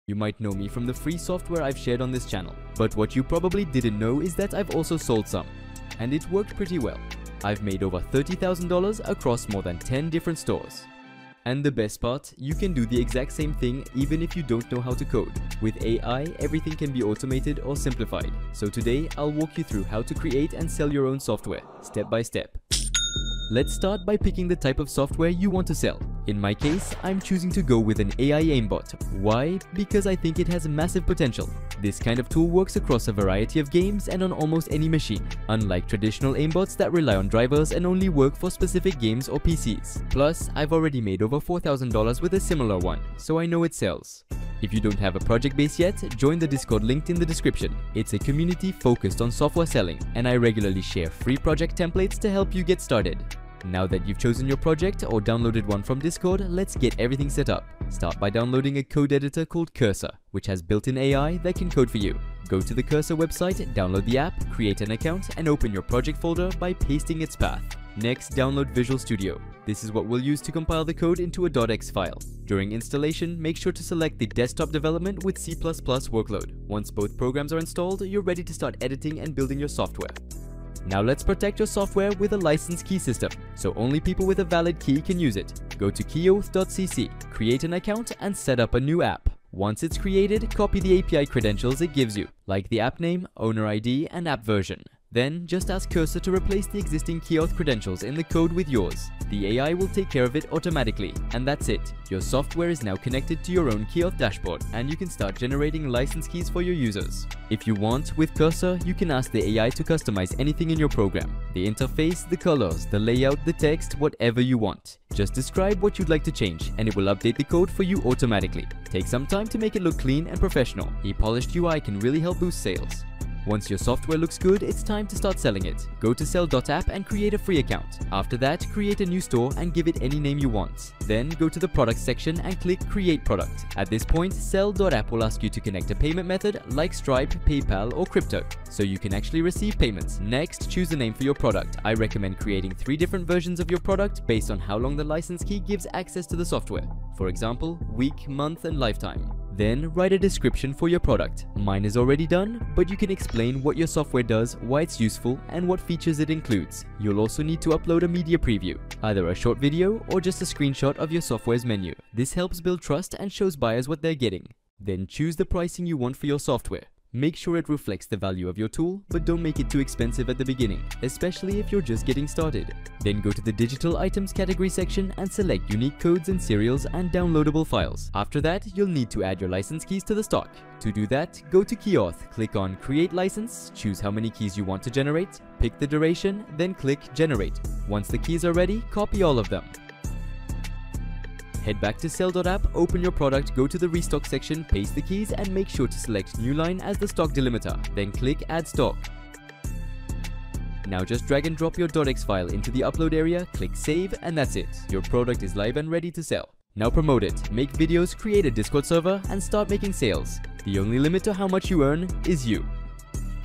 How I Made $30,000 Selling AI Software (Without Coding)
Learn how to create, protect, and sell your own software, without writing code.

💬 Get help, updates & support directly in the Discord.
👥 Meet other creators building and selling AI-based tools.

🧠 Whether you're launching your first product or just exploring, you're in the right place.